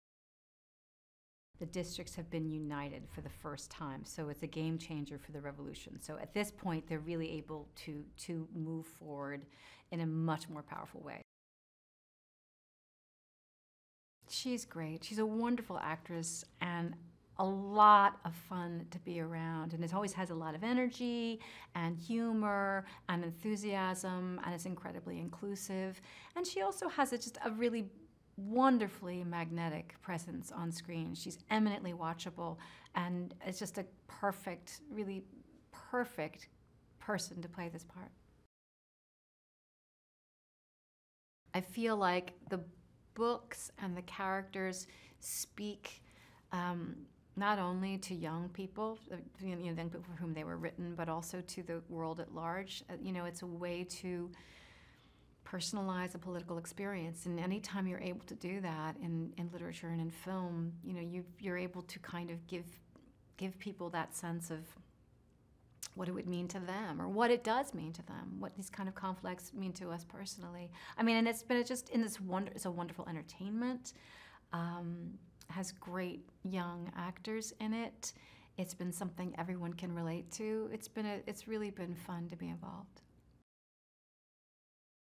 The Hunger Games: Mockingjay - Part 2 Interview - Julianne Moore (2015) - Movie HD
As the war of Panem escalates to the destruction of other districts by the Capitol, Katniss Everdeen, the reluctant leader of the rebellion, must bring together an army against President Snow, while all she holds dear hangs in the balance.